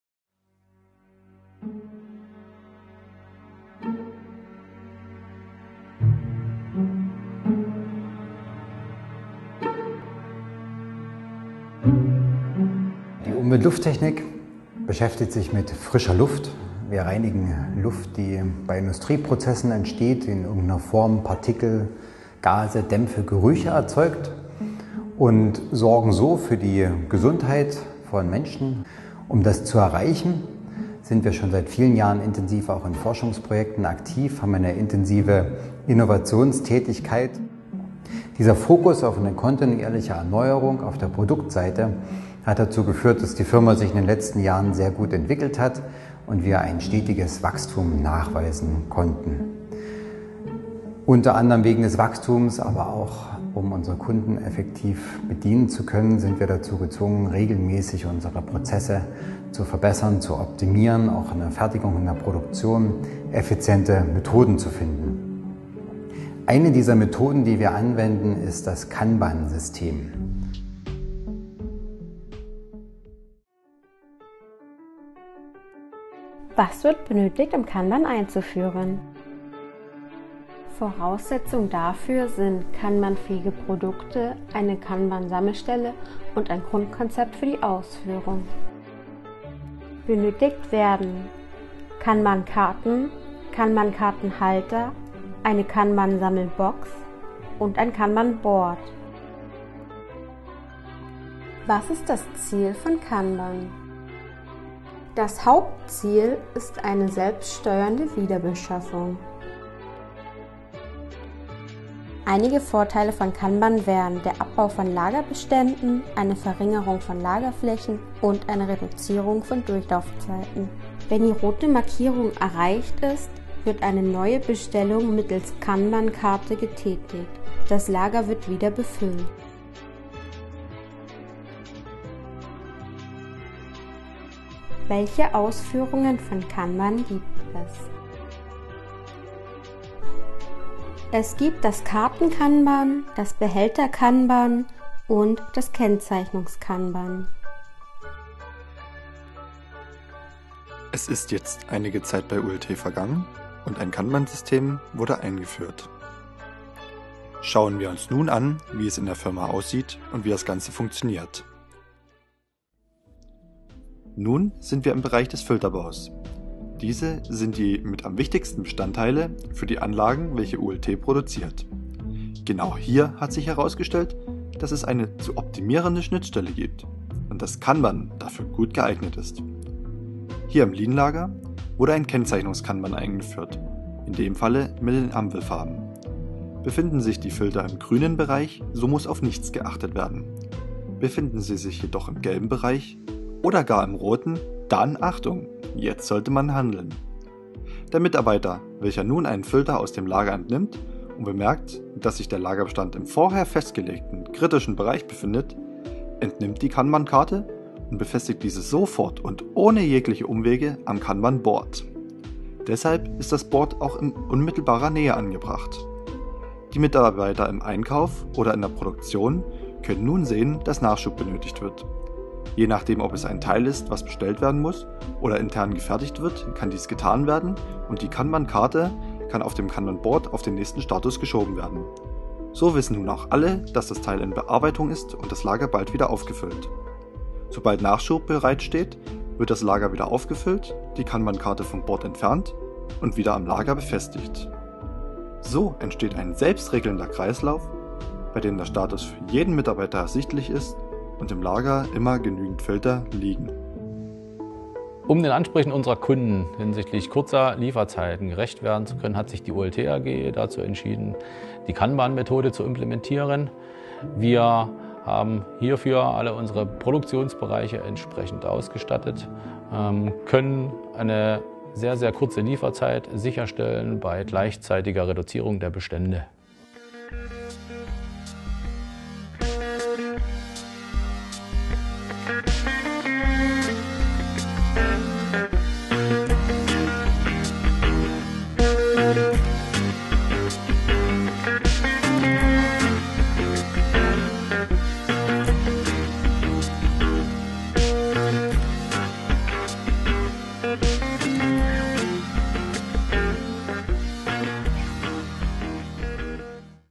Studierende erklären | Methoden zur Produktionsoptimierung | Kanban am Beispiel der ULT AG
Wie kann man Produktionsbestände und -abläufe für jeden Mitarbeiter auf einen Blick erkennbar machen? Studierende des Studienganges „Wirtschaftsingenieurwesen“ der Hochschule Zittau/Görlitz haben sich die Methode "Kanban" im Löbauer Unternehmen "ULT - Umwelt-Lufttechnik" angeschaut und geben die Antwort.

In der Lehrveranstaltung „Fertigungswirtschaft“ erarbeiteten Studierende – in Gruppenarbeit zu vorgegebenen Themen der Produktionsoptimierung – in einem dreistufigen Prozess zunächst Präsentationen und Übungen für ihre Kommilitonen. Darauf aufbauend wurden nach einem Peer Assessment und Feedbacks durch ein interdisziplinäres Dozententeam sogenannte Micro Teaching Videos (MTVs) durch Studierende produziert.